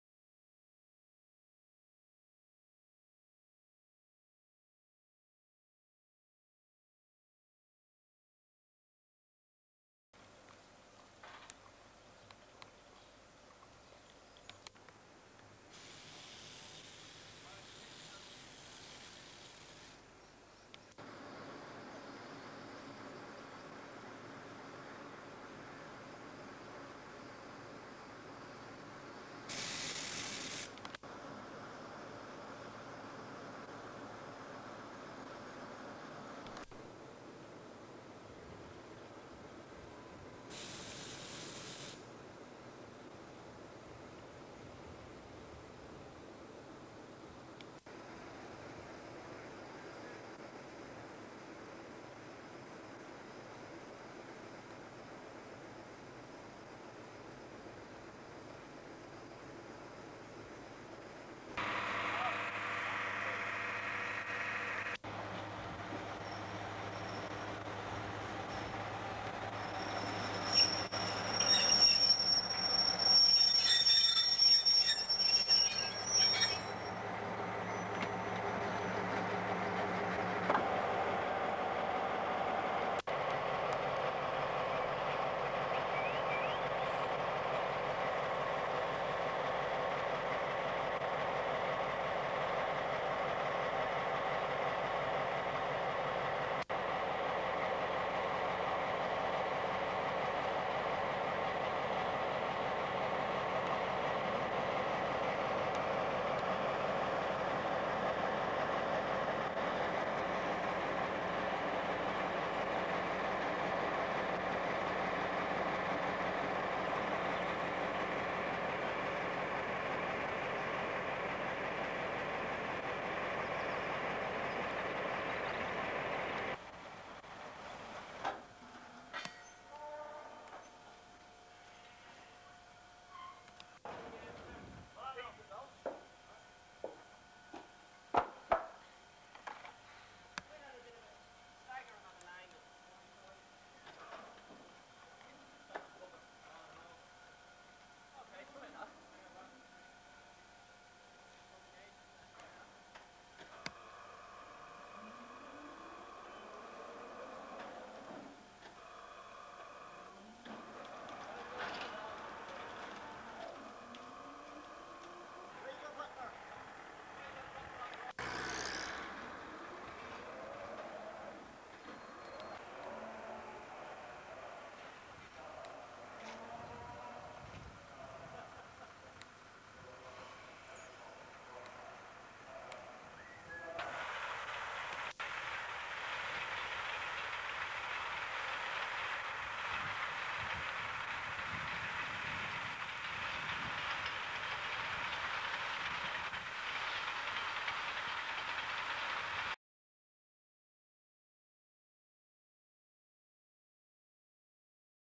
Westrail Activities May 1989
West Rail scenes filmed in Tuam and Claremorris in May 1989.